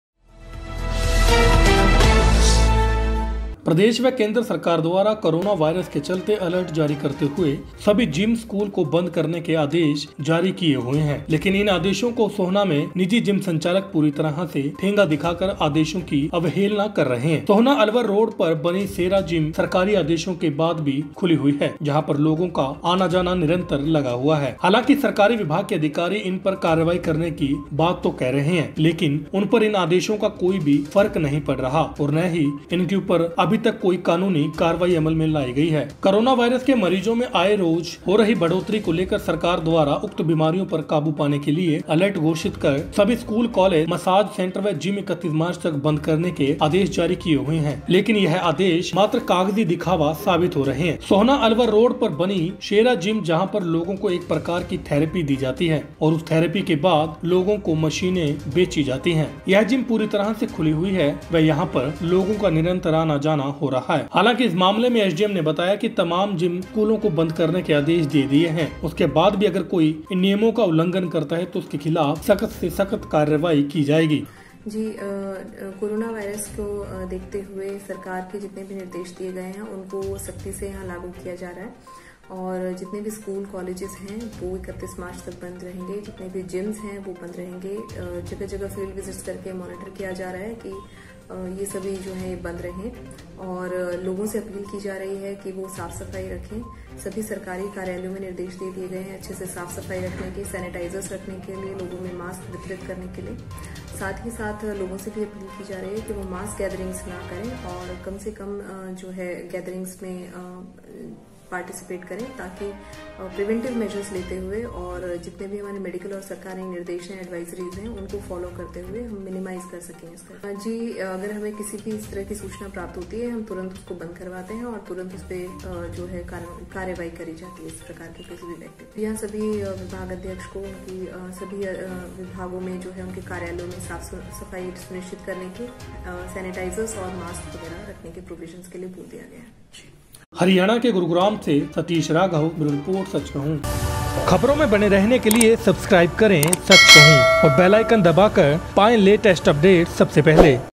Sohna || सरकारी आदेशों को ठेंगा दिखा रहे निजी जिम संचालक
प्रदेश व केंद्र सरकार द्वारा कोरोना वायरस के चलते अलर्ट जारी करते हुए सभी जिम स्कूल को बंद करने के आदेश जारी किए हुए है । लेकिन इन आदेशों को सोहना में निजी  जिम संचालक  पूरी तरह से ठेंगा दिखा कर आदेशो की अवेहलना कर रहे हैं ।सोहना अलवर रोड पर बनी सेरा जिम सरकारी आदेशों के बाद भी खुली हुई है जहां पर लोगों का आना-जाना  लगा हुआ है ।हालांकि सरकारी विभाग के अधिकारी इन पर कार्रवाई करने की बात तो कह रहे हैं लेकिन उन पर इन आदेशों का कोई भी फर्क नहीं पड़ रहा ओर ना ही इनके ऊपर अभी तक कोई कानूनी कार्यवाही अमल में लाई गई है।
बाइट:-चिनार चहल एसडीएम सोहना।